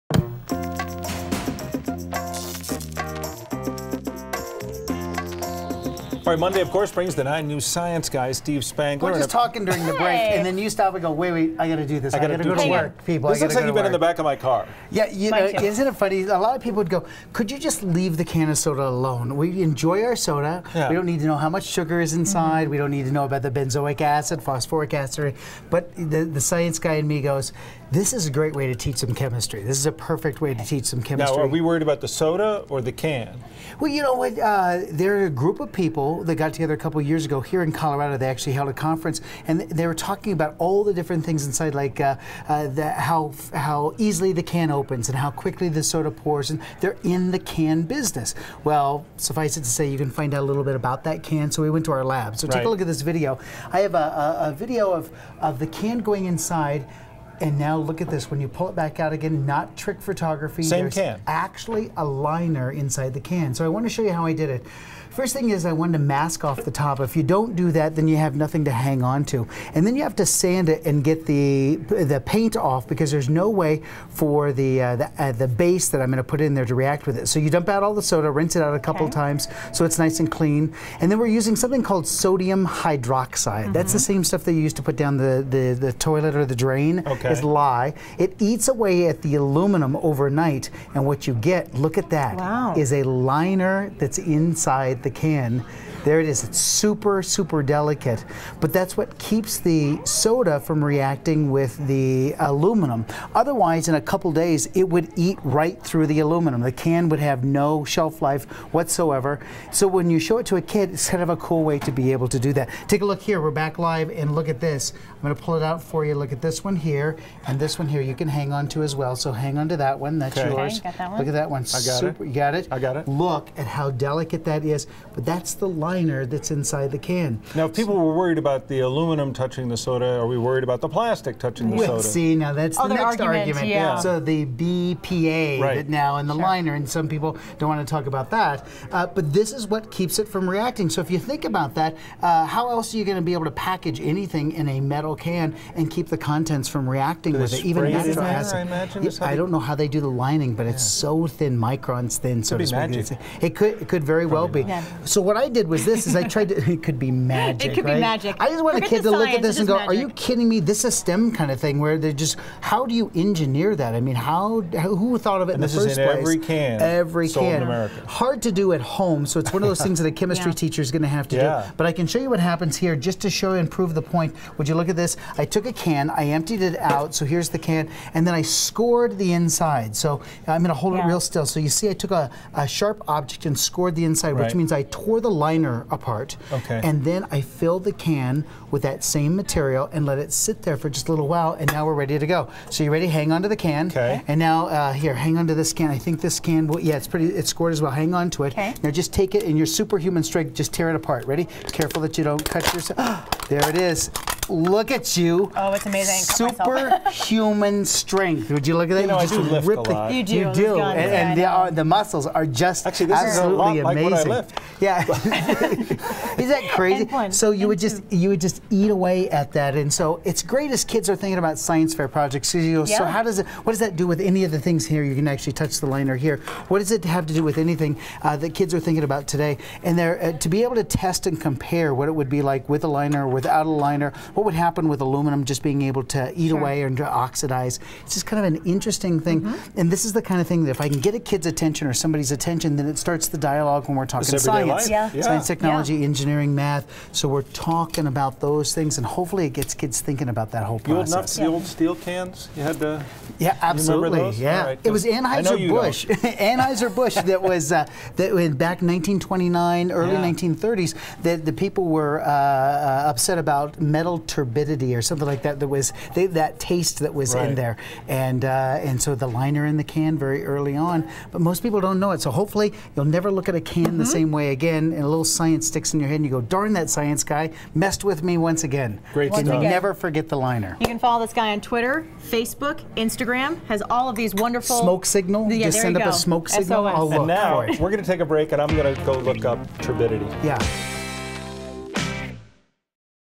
Inside a Soda Can - Hidden Liner
You won’t believe what our science guy Steve Spangler found inside a soda can. In fact, it’s inside every soda can and it protects the aluminum from reacting with the soda. Steve takes us inside his lab for a chemistry lesson you won’t forget